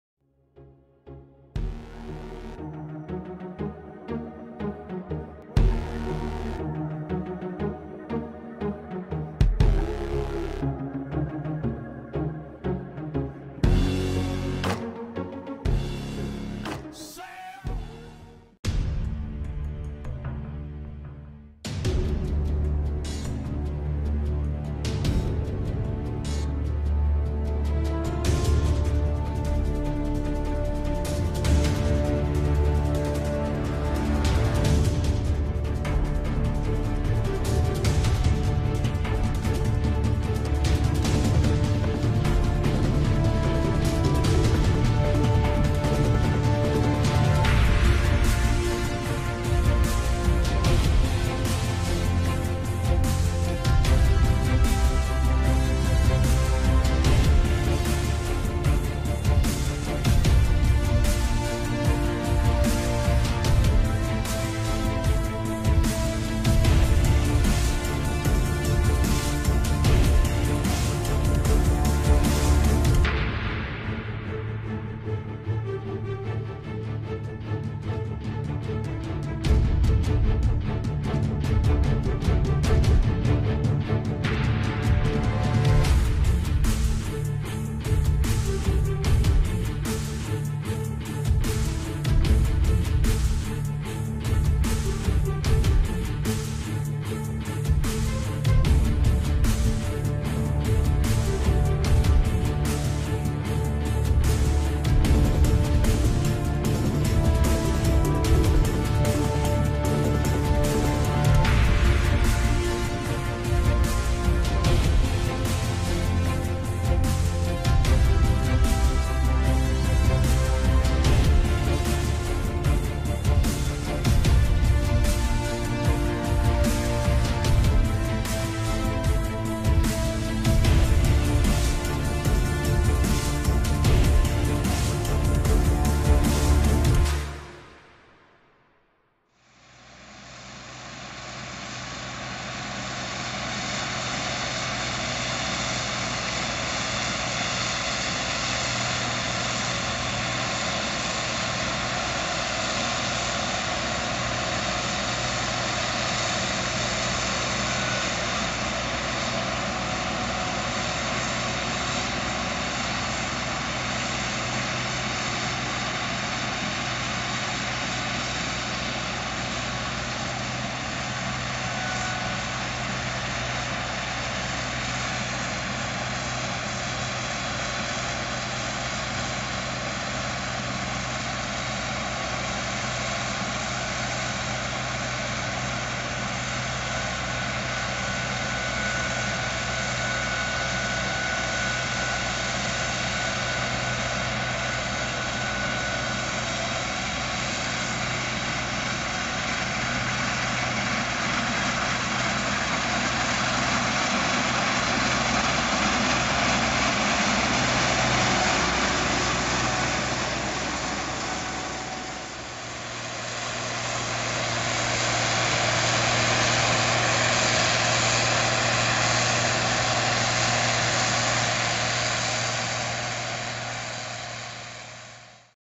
*CASE IH* Trebbiatura soia 2014
La mietitrebbia 2166 Axial Flow di Case IH ha una una potenza di 215cv, e lavora accoppiata ad una barra pieghevole Cressoni da 5 metri.